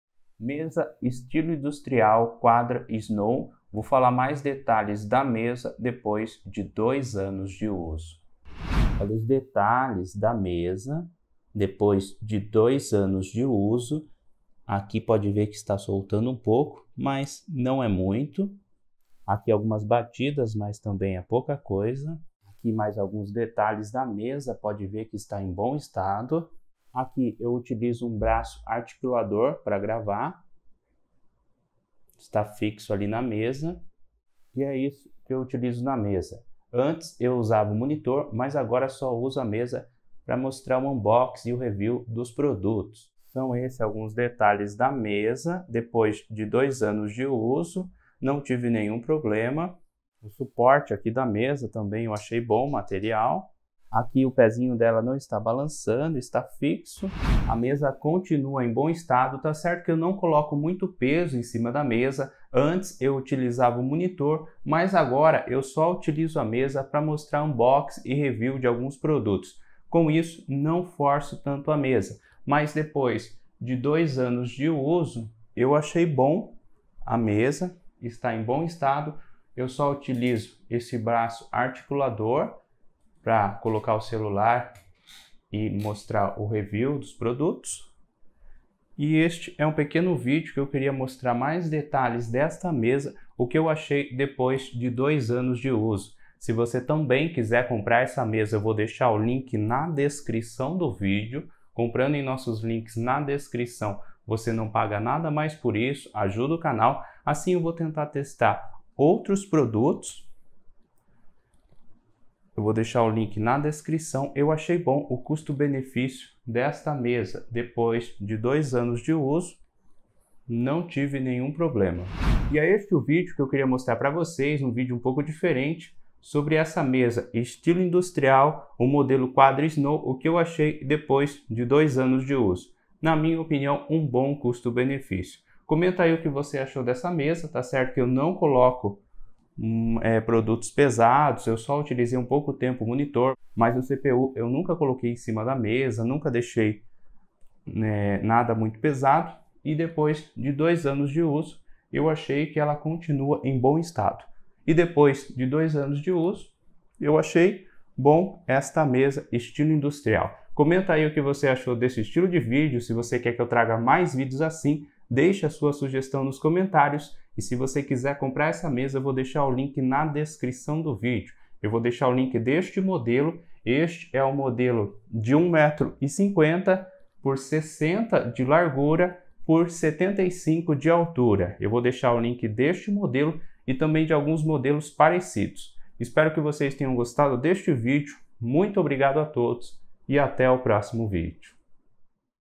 MESA Estilo INDUSTRIAL KAUDRA SNOW depois de 2 ANOS
🛒 Mesa Estilo Industrial Kuadra (Amazon)

🛒 Outros Mesas (Amazon)

(00:00) Introdução
(00:08) Detalhes da mesa
(00:57) Qualidade da mesa
(02:02) Minhas impressões